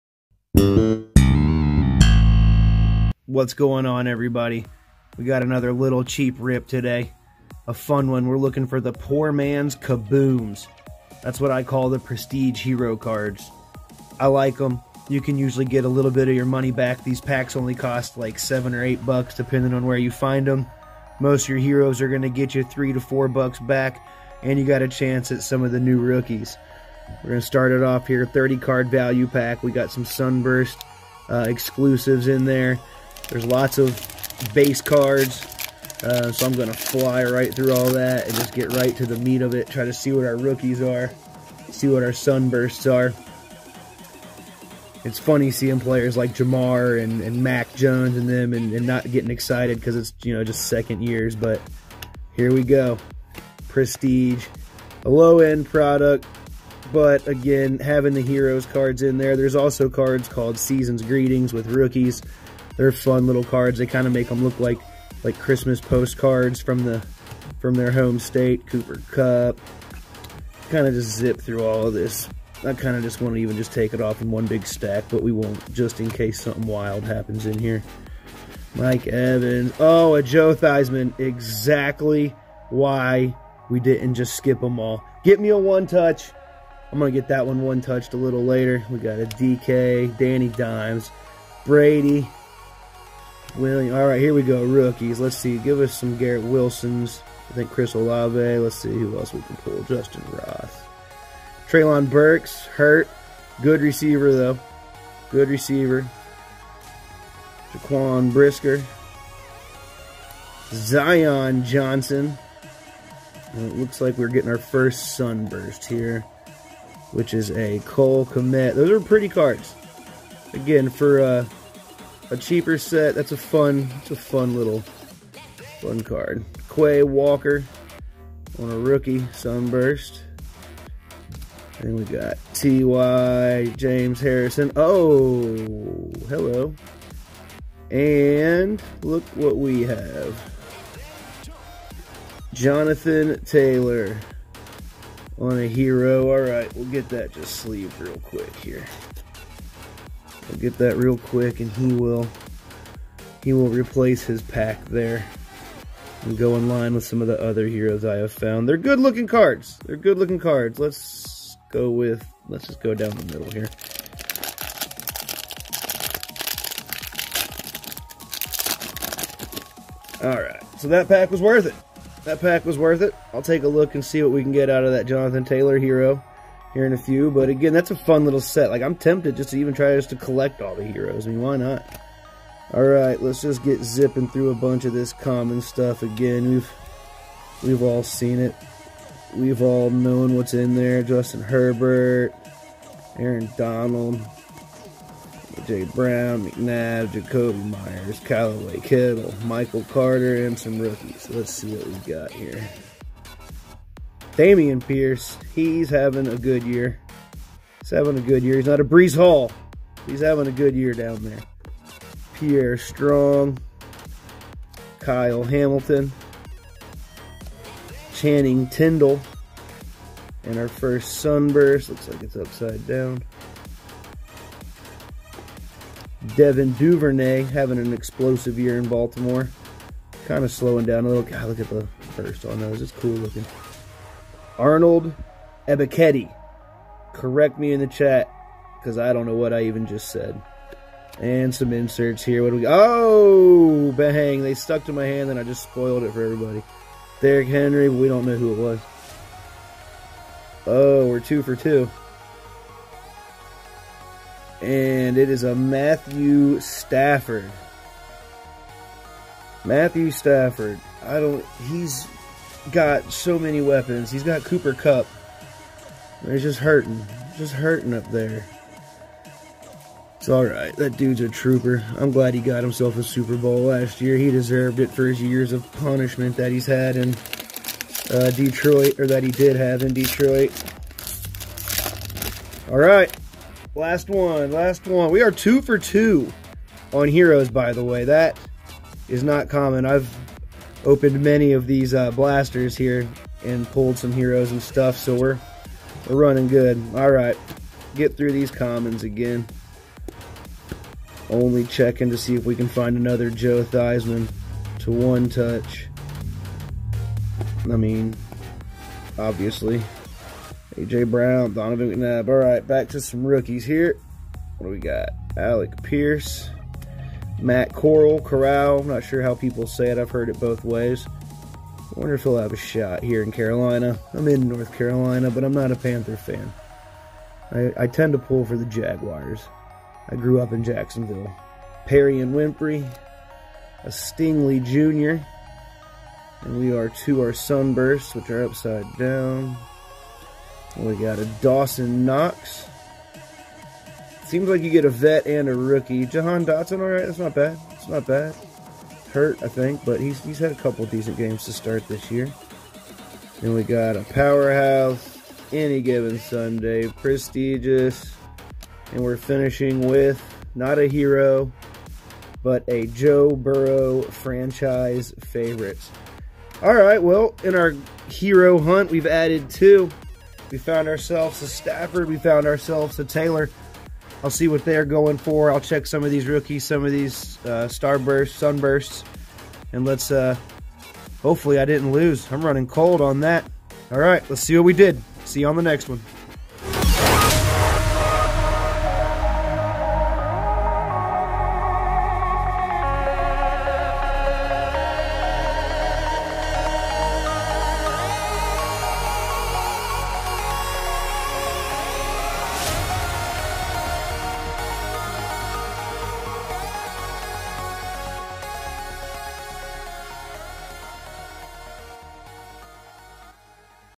HB BREAK 3: 2022 PANINI PRESTIGE, 30-card Walmart Value Packs; HERO HUNTING!
just like the title says, we are HERO HUNTING!

Prestige is by no means a top shelf, sought after product, however, it is a cheap rip to hunt for fun inserts and new rookies.

I call Heroes cards "Poor Man's Kabooms" which might be why I like them so much, anyways... ENJOY THE RIP!

Let me know what you think about Prestige and Hero Hunting in the comments!